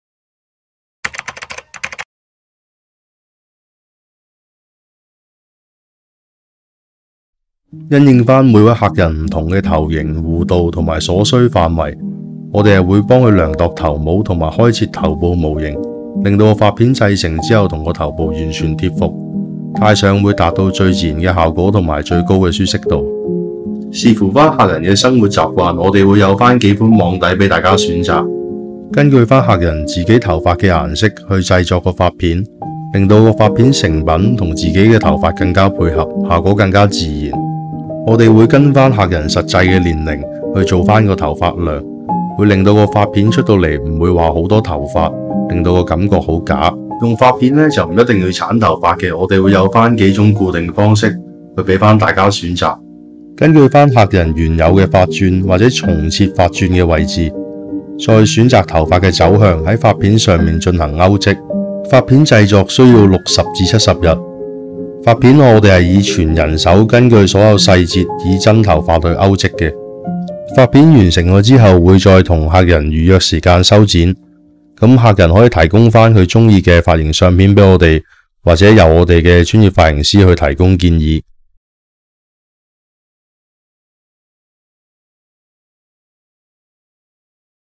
香港愛心連線『尋找品牌的故事』HAIR Recovery復原法
感謝 『HAIR Recovery復原法 - 手工編織假髮 』 參與及支持香港愛心連線的2020青少年實習及就業活動『尋找品牌的故事』

片中會幫助客人量度頭模和開設頭部模型，讓髮片製成後與頭部完全貼服，增強舒適度。根據客人的實際年齡製造頭髮量，避免不自然。製作需要60至70日，以全人手真髮勾織...